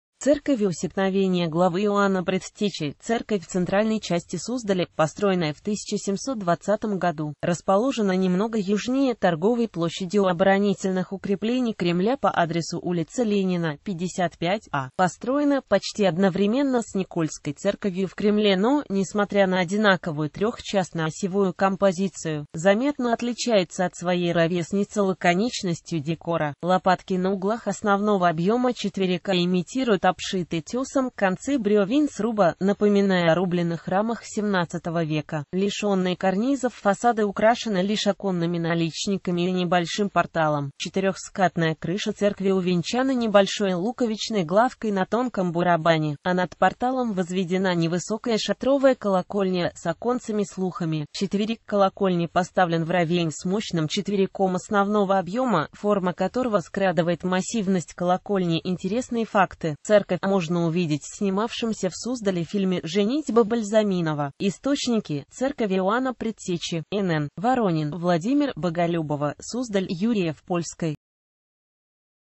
Церковь Усекновения Главы Иоанна Предтечи (Суздаль)
Церковь Усекновения главы Иоанна Предтечи — церковь в центральной части Суздаля, построенная в 1720 году.Расположена немного южнее Торговой площади у оборонительных укреплений Кремля по адресу ул.Ленина, 55а.Построена почти одновременно с Никольской церковью в Кремле но, несмотря на одинаковую трёхчастно-осевую композицию, заметно отличается от своей ровесницы лаконичностью декора.